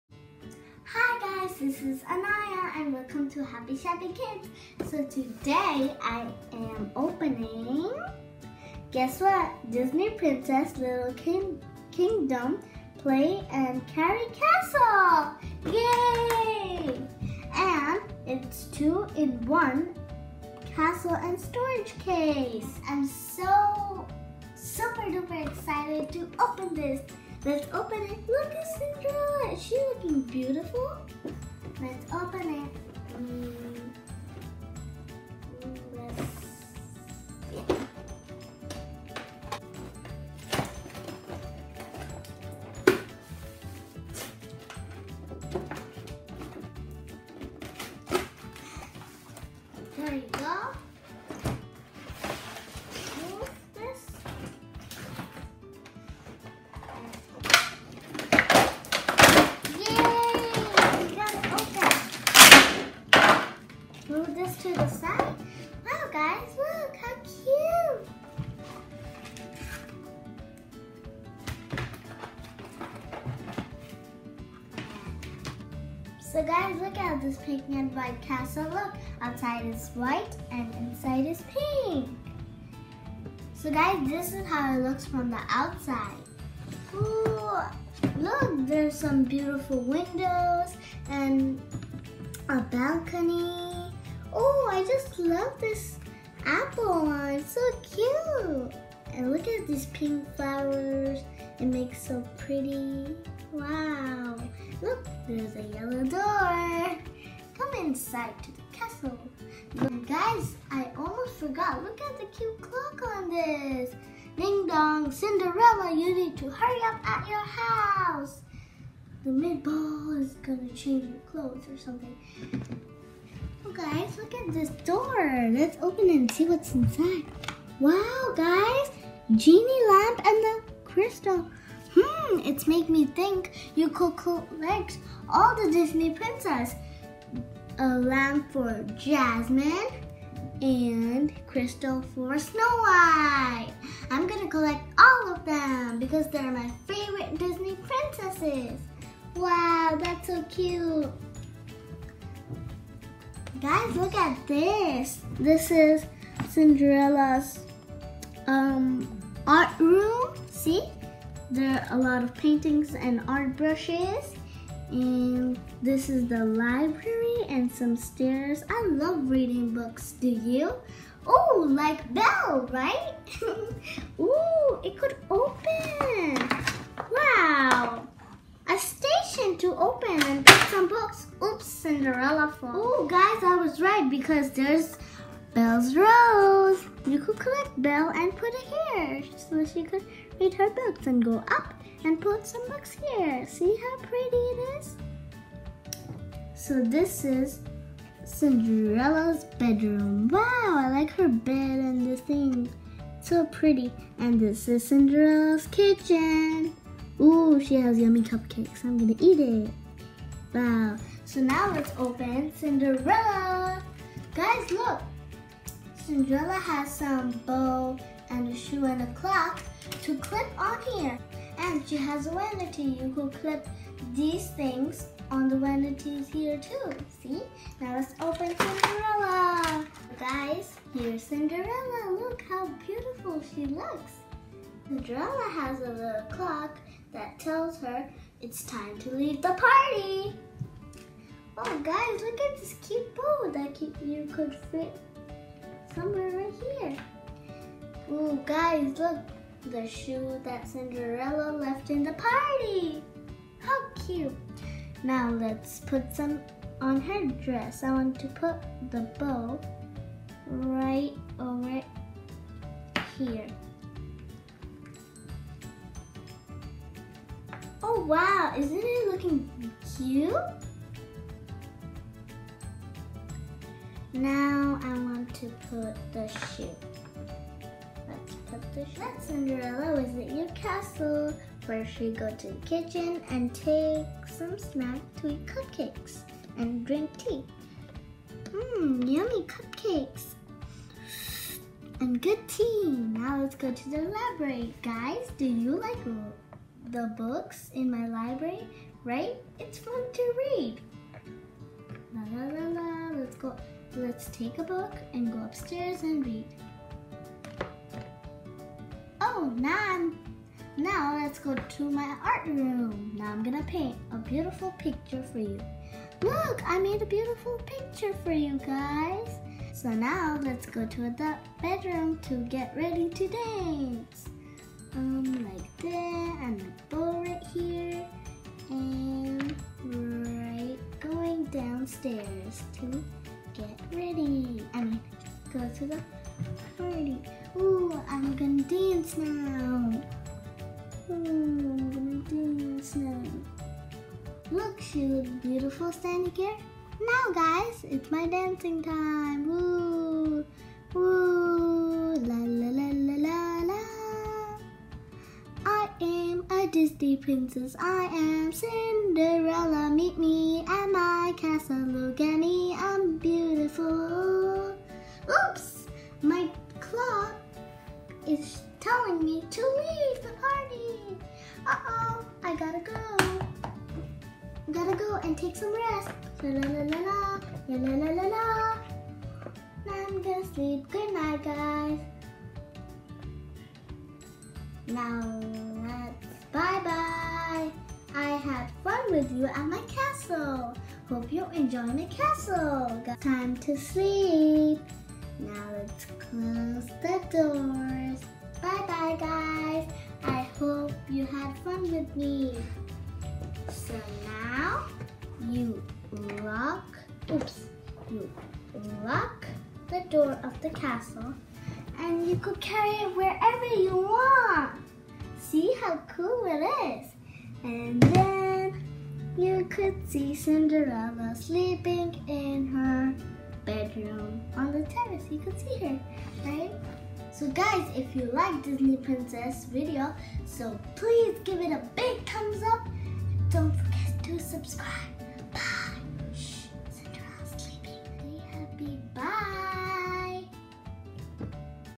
Unboxing Disney Princess Play & Carry Castle And Pretend Play
Hey guys, in this video I unbox a beautiful Disney princess play and carry castle. It’s really cool and fun to play with it and the best part you can carry it with you wherever you want.

Stay happy 😊😊😊